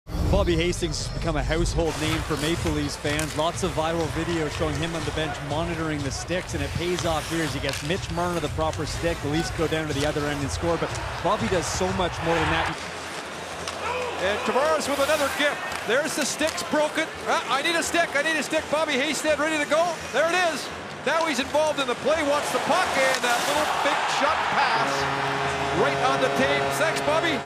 Marner & Hastings stick together 🏒
Mitch Marner and Leafs Equipment Manager Bobby Hastings combine again, as Hastings provides Marner a new stick on Wednesday night, leading to an assist. The two had previously connected on a similar moment in 2018, with Hastings' quick stick change leading to another Marner assistl vs. the Sharks

Feeling social?